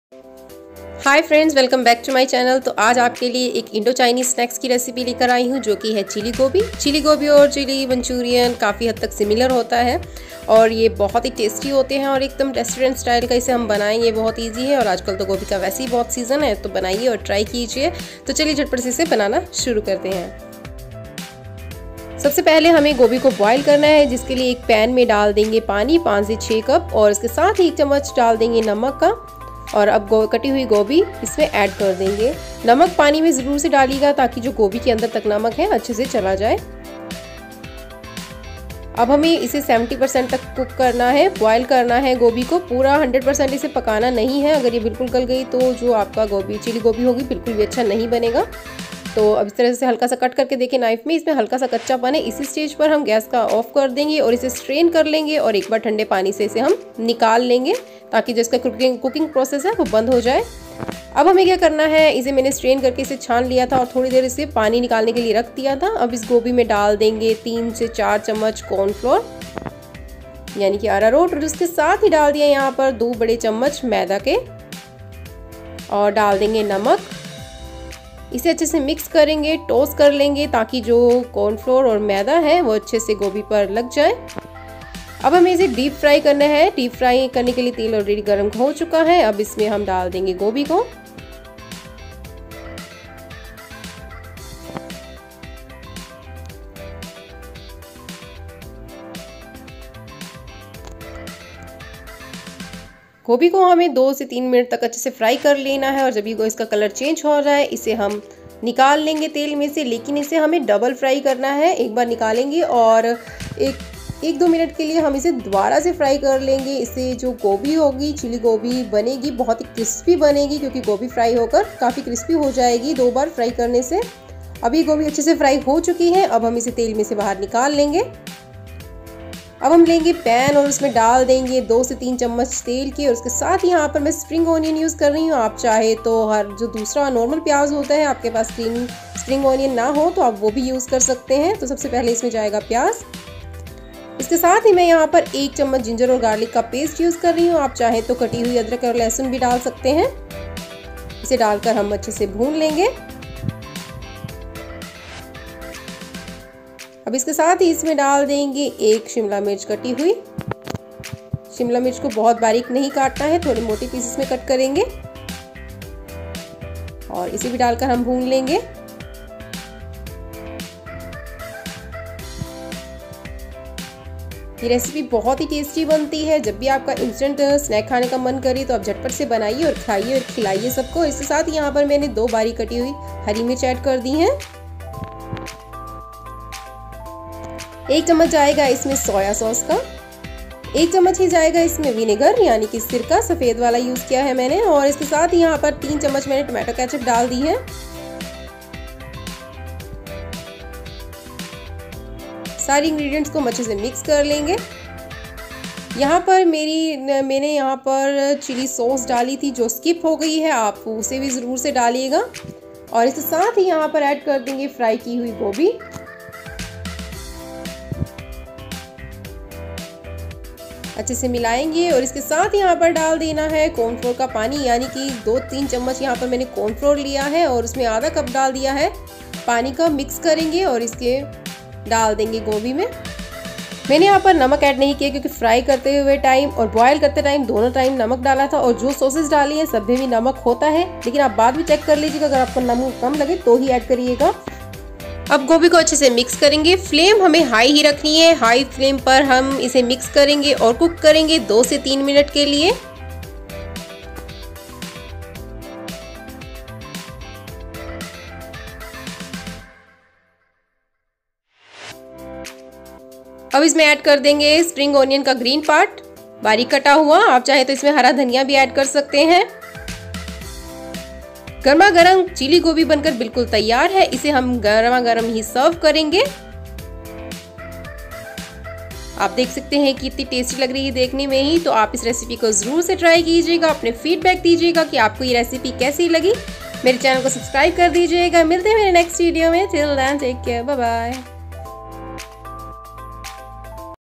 Chilli gobi recipe/Chilli gobi kaise banate hai
Today I will make chilli gobi (cauliflower) (chilli phool kobi) recipe.So learn the step by step procedure of this delicious dish made by me. This is chinese recipe you will definitely like this. so give a try and leave your feedback 🥰